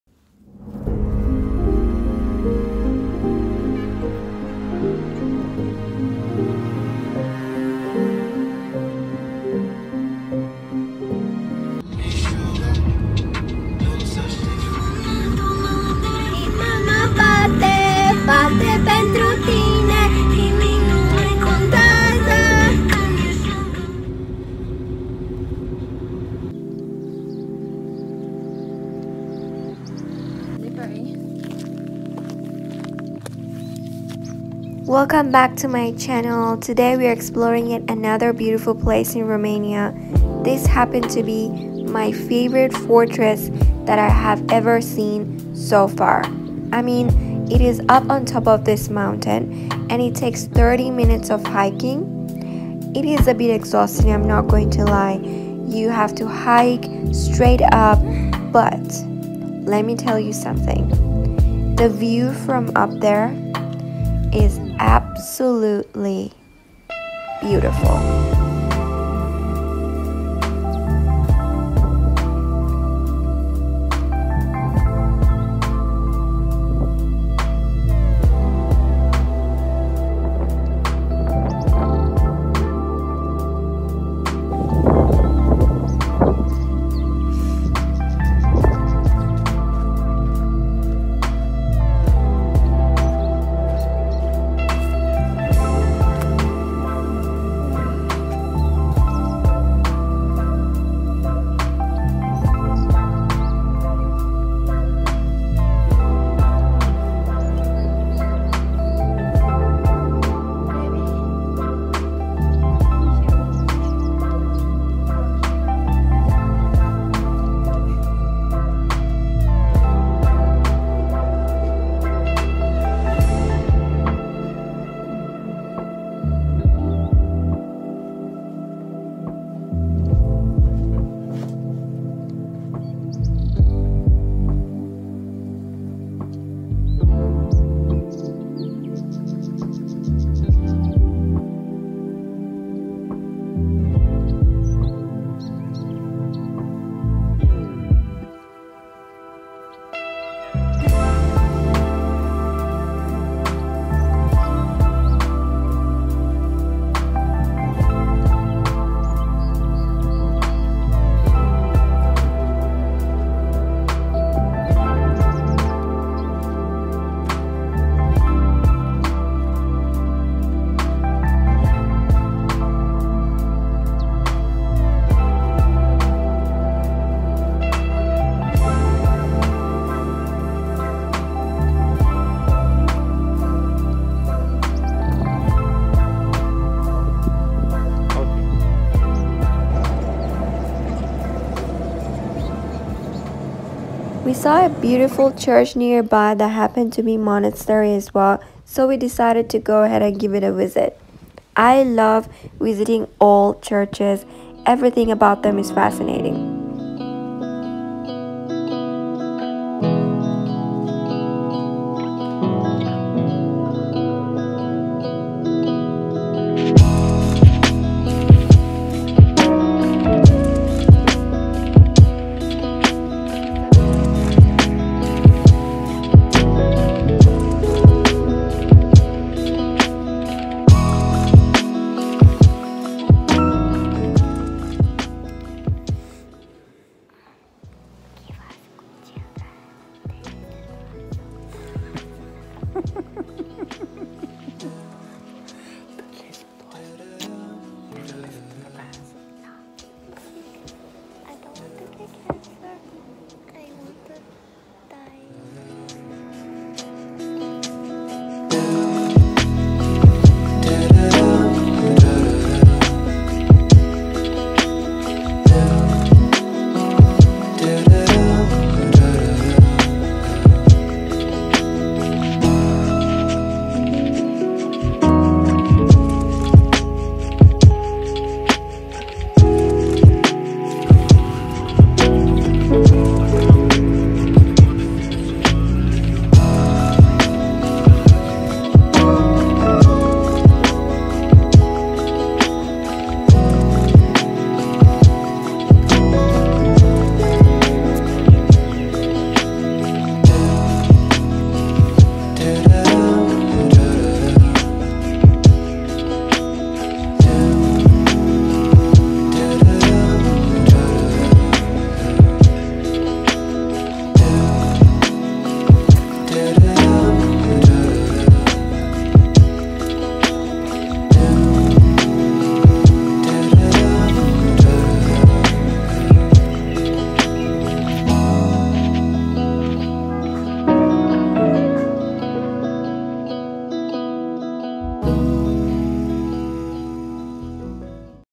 My favorite fortress in Romania | Cetatea Șoimoș | Lipova- Arad
Hi friends.
Here is another video of Exploring Romania, This beautiful fortress is located in Lipova. it has 30 mins hiking up with moderate difficulty. The view from up is beautiful.
 Let me know which fortress is your favourite in Romania ?

TT : @thelifeofyelna.

Music in this video are owned by their respective artists .